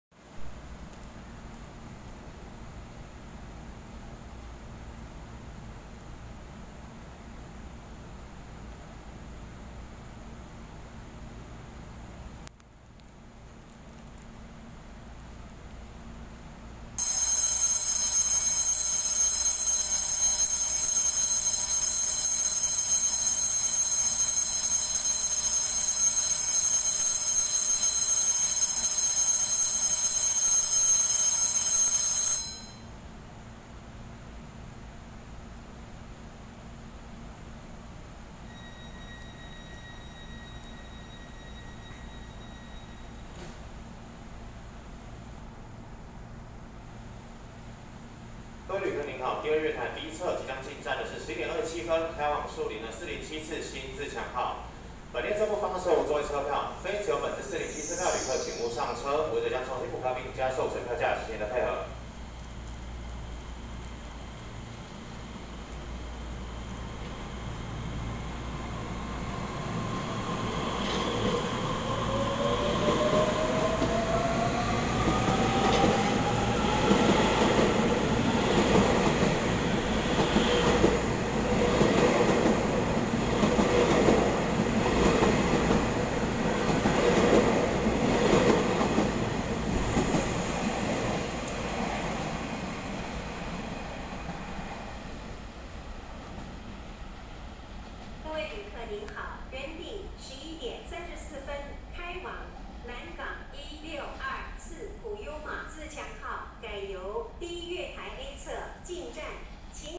222次新自強號城際列車EMU(3000)型板橋站出站
222次新自強號 沿途停靠（樹林，板橋，台北，松山，南港，宜蘭，羅東，花蓮，吉安，志學，壽豐，林榮新光，鳳林，光復，瑞穗）